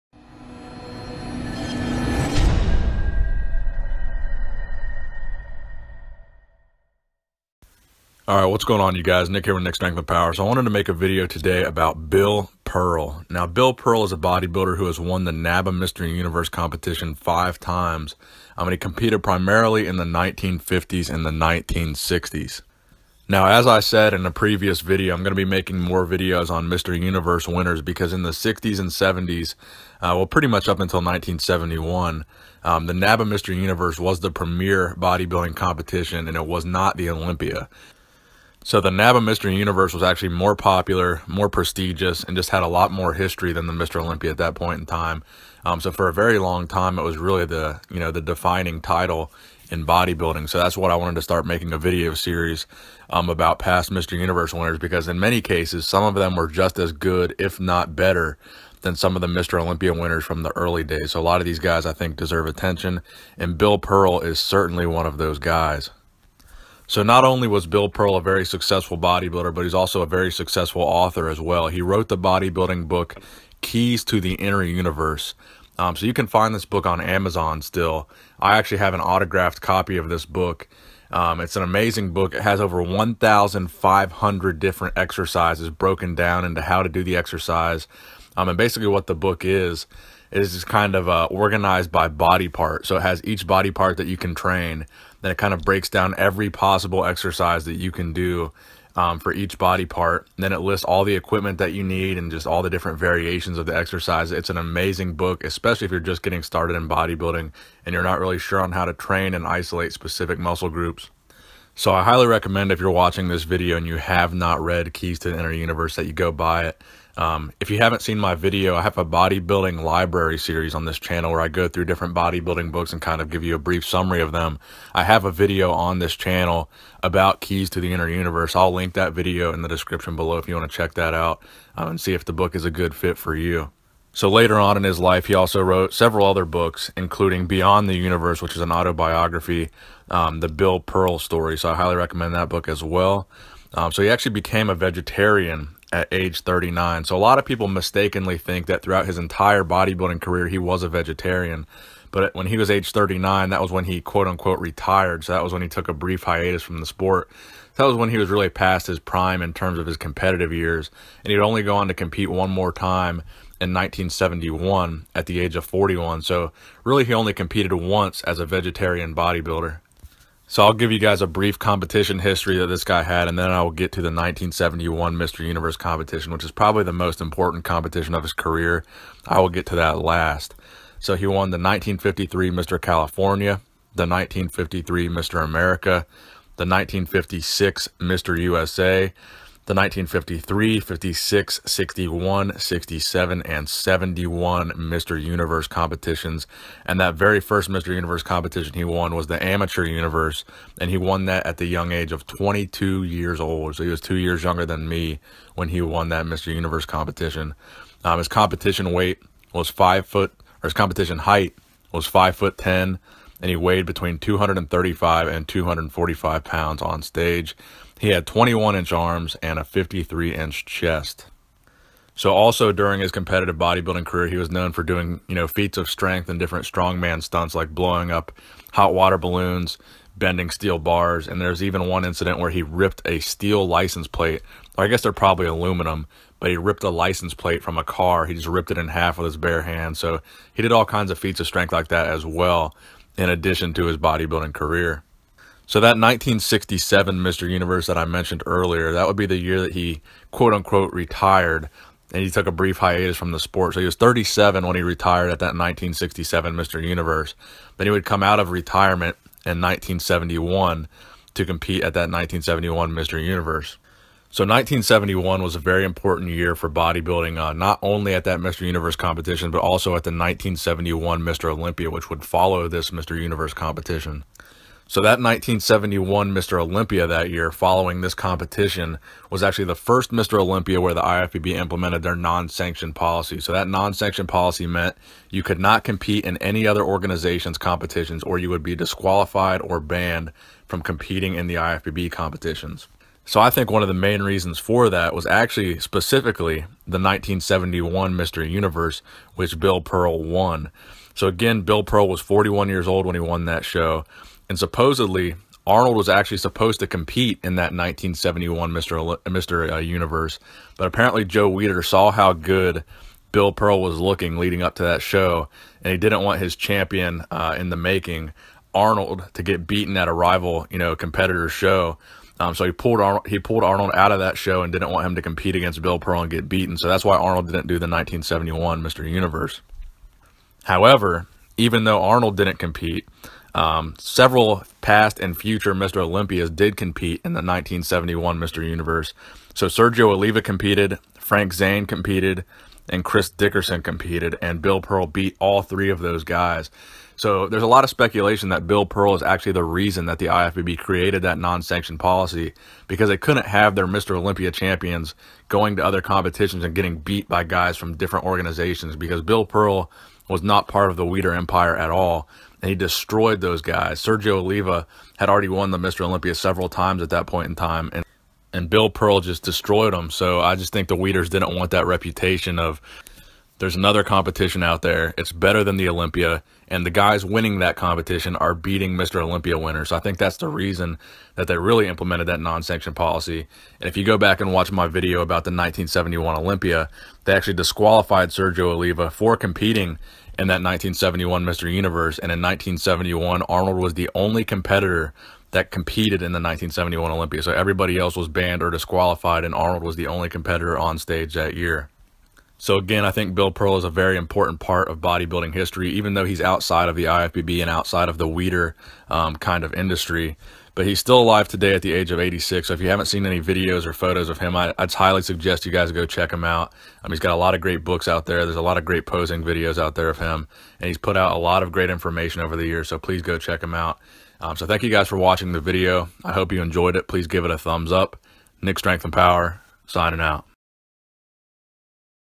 Bill Pearl - The Mr. Olympia Slayer
In the 1971 Mr. Universe Bill Pearl defeated Sergio Oliva, Frank Zane, and Chris Dickerson. All were past and future Mr. Olympia champions. Bill looked so good that Arnold was pulled from the show by Joe Weider so Pearl wouldn't defeat him. Some say Pearl was the reason for the IFBB's "Non-Sanctioned" policy.
*Interact with me on social media*